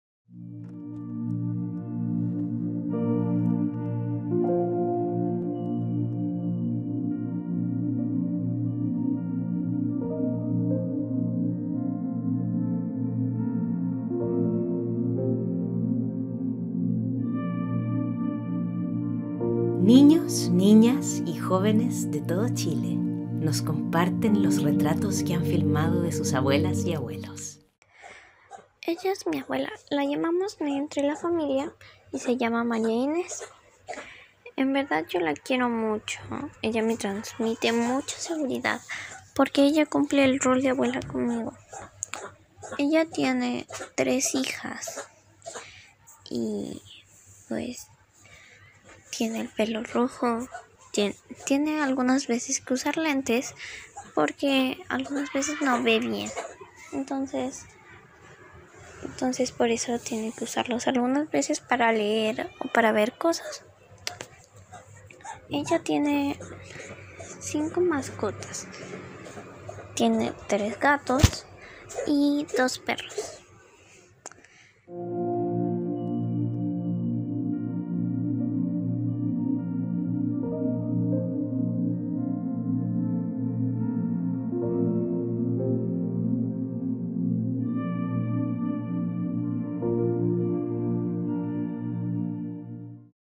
Emilia Soto | Retratos de los abuelos y las abuelas
Niños, niñas y jóvenes de todo Chile nos comparten los retratos que han filmado de sus abuelas y abuelos en distintas zonas y actividades.